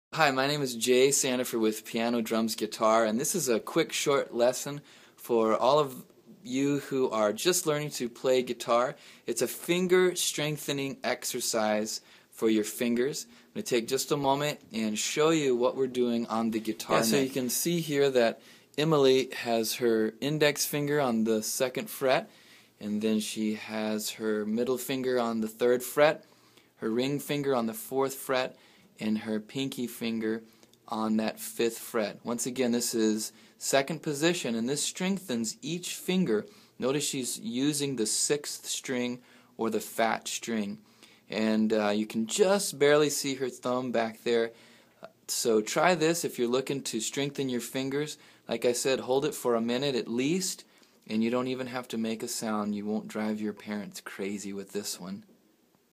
Finger Strength Exercise for Beginner Guitar Students
Try this simple exercise to strengthen your fingers. Also, begin to develop muscle memory.